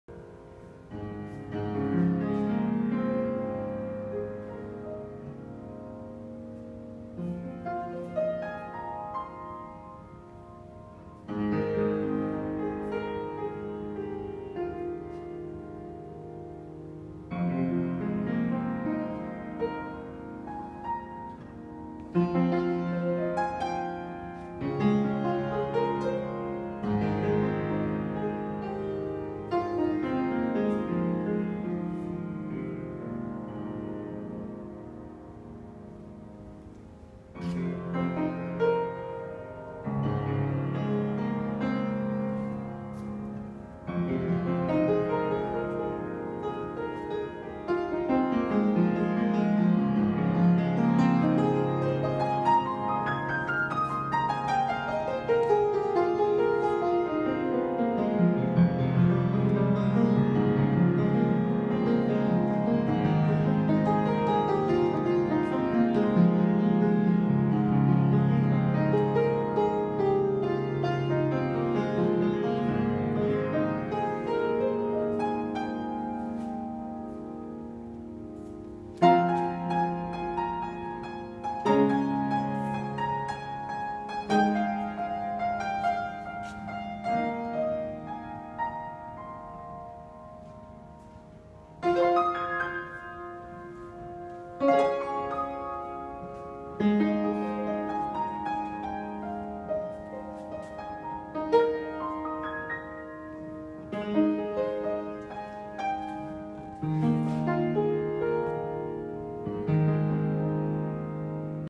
Canvas mural project with an antique baby grand piano playing in the background
Dropping off these canvas paintings and we found a baby grand piano and Martin sat down and played while I panned across the paintings, these are a total of 12 feet wide, 2 sections, each 6 ft wide.I had to tie these onto the roof of the car to deliver them. Perhaps you will see more of them soon in public places.